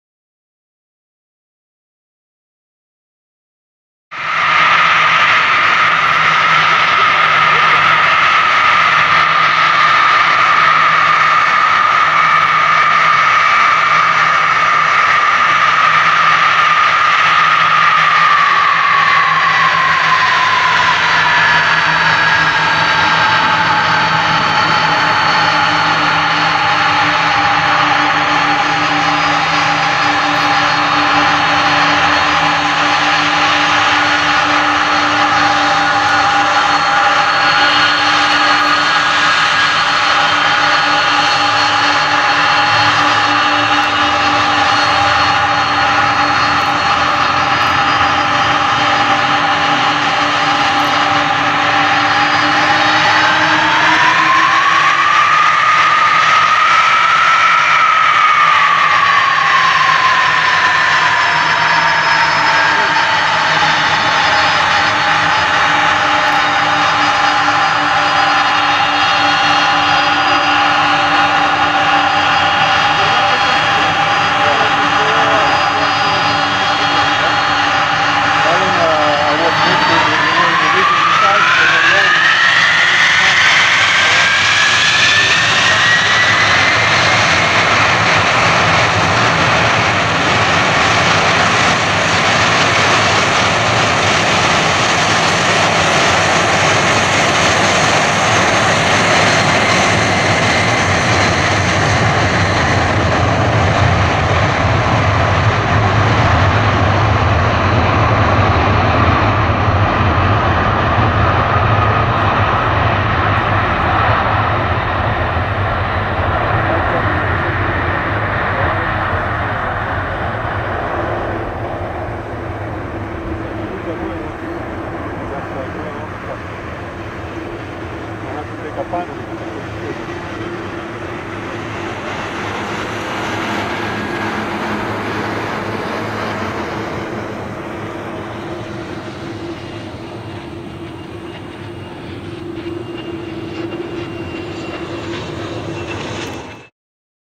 Martinair Holland MD-11F [PH-MCU]
Martinair Holland MD-11F departing on 9 from Miami International Airport (MIA) on January 31, 2016